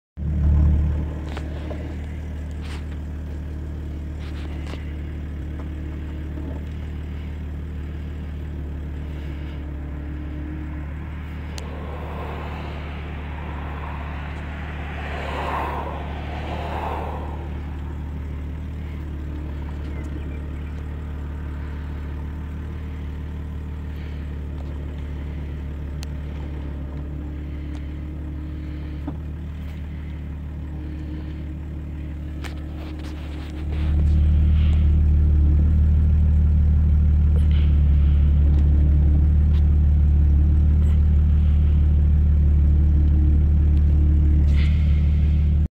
Easy steering XJ
Swapped a ZJ steering box into my 99 XJ in pursuit of faster steering. It does indeed turn faster and sharper, but... It will also steer itself. Right before the clip starts it began turning to the left of its own accord, but I only caught the tail end of that on film. This was the first and only time I've seen this failure in a traditional hydraulic power steering system.